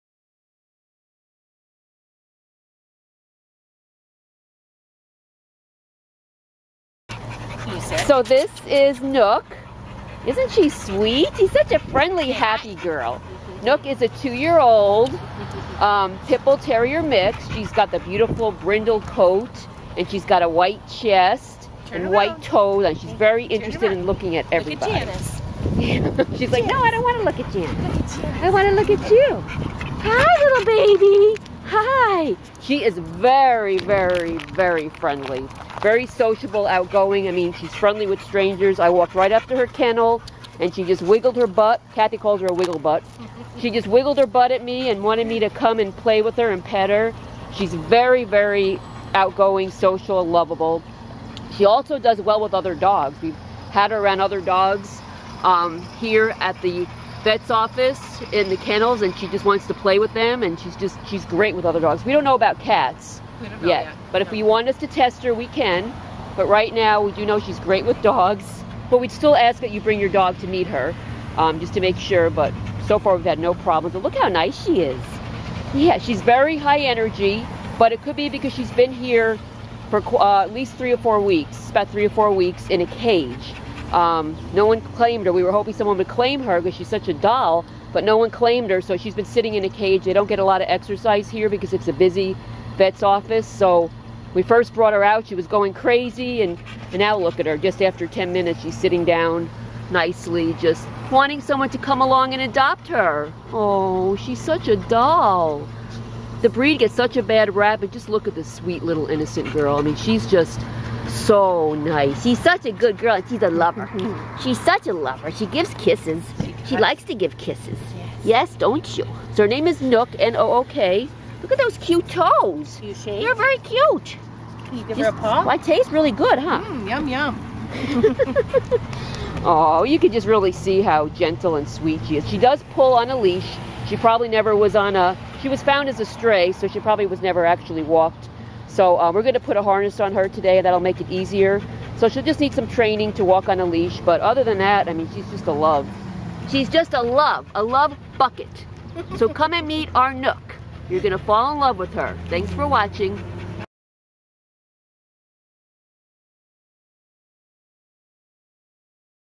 Nook
Meet the LOVABLE Nook!  Nook is a 2 year old female Pit Bull Terrier mix.  She is a beautiful brindle coloring with a white chest and toes. Actually in looking at one of her photos she looks tiger striped. Nook is one of those lovable dogs that just starts wiggling around and getting so excited as soon as you approach her kennel. She is such an extremely friendly and OUTGOING dog. She is NOT shy. She LOVES people, she is so happy and just wants to please. She does GREAT with other dogs. When she goes by their kennels she stops and tries to get them to play with her. So she would actually like a friend.  We have not cat tested her yet. She will need some training to walk on a leash, probably had never been on one. A fenced in yard would be great for her to burn off her energy. But as you can see by her video, she can be very calm once she burns off that initial burst of excitement.  She has a sweet and loving temperament. You have to meet her!!